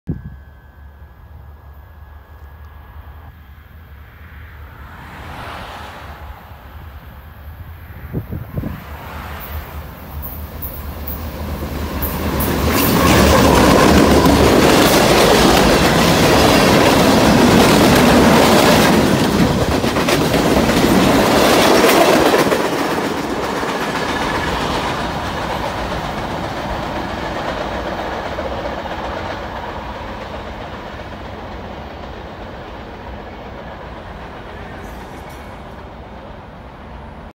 UP Salinas Hauler passes MP 61.40
On my way home from Santa Clara, I noticed a green signal showing for a northbound train at MP 61.40 between CP Perry and CP Coyote, and it turned out to be the UP Salinas Hauler with UP GP60’s #1095 and #1039 pulling 9 cars including a heavily graffiti tagged tank car at the rear with an amazing sunset in the back for great lighting. If you look carefully you’ll notice that the crew was operating the train from the second unit.